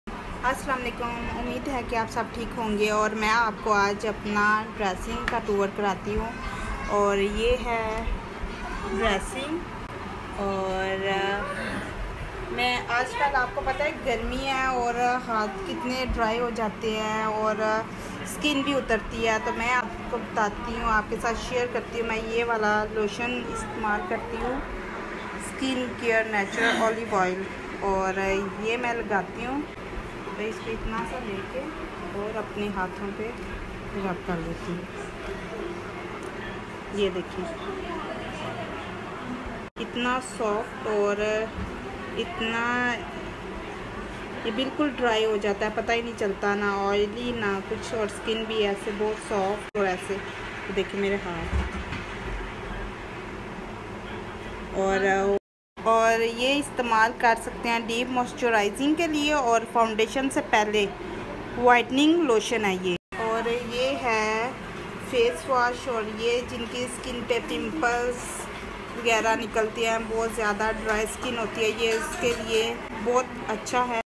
Skin care vlog 🧴
Skin care vlog # me share Jin ki  skin dry  ho jati hai. Un k liya kuch produced hai. Jo main ny ap ko detail main batai hai.